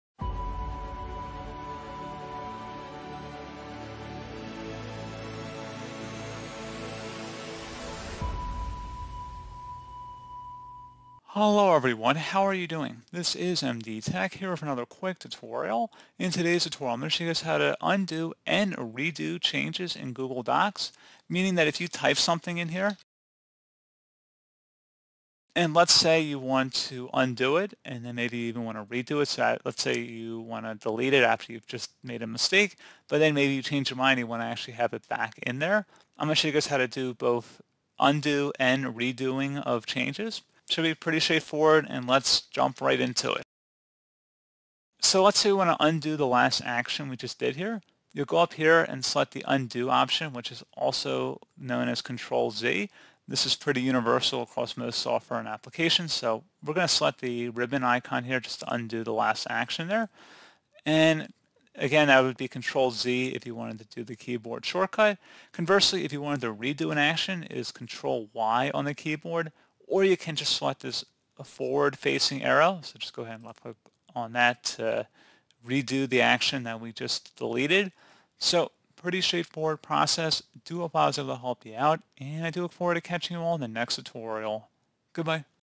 How to Undo and Redo Changes in Google Docs [Guide]
You don’t need to be afraid of making a mistake in Docs, because you can use the Undo feature to erase your actions. The undo and redo commands are very useful for working with text in a document.